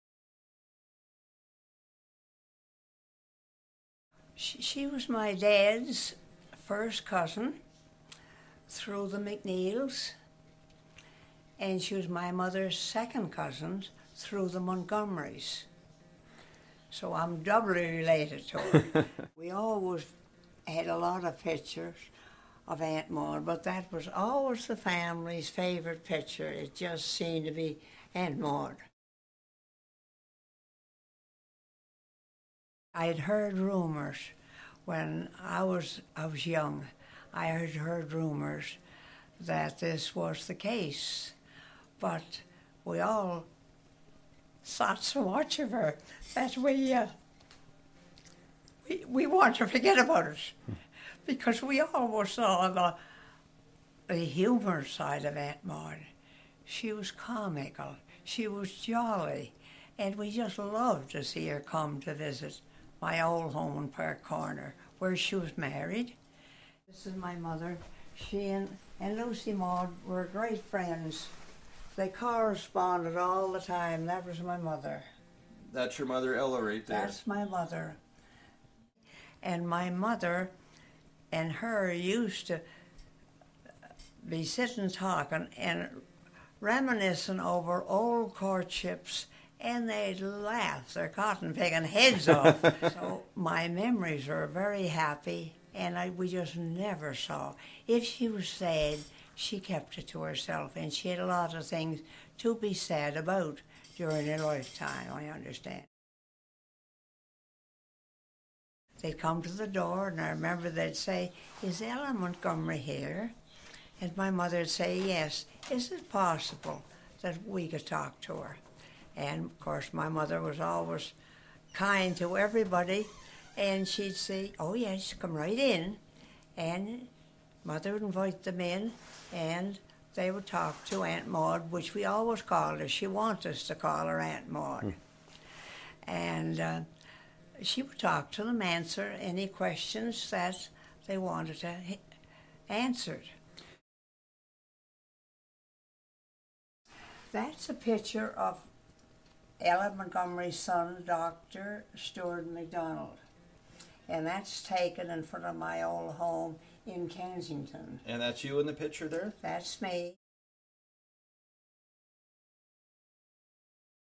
MacLeod interview about L.M.Montgomery's suicide
L.M.M's relative, Georgie Campbell MacLeod's interview.

[Fond memories of Lucy Maud]
30/09/08
Reports that L.M. Montgomery took her own life were devastating to relative Georgie Campbell MacLeod
JIM BROWN
The Journal Pioneer (Summerside)

To Georgie Campbell MacLeod, Lucy Maud Montgomery was more than just a writer.
The author of "Anne of Green Gables" was a cherished member of the family. And Montgomery was a frequent visitor to her family's Park Corner home.
So when Montgomery's granddaughter recently wrote a column saying the author took her own life, MacLeod was devastated.
"My heart was broken," said MacLeod, who doesn't believe Montgomery committed suicide.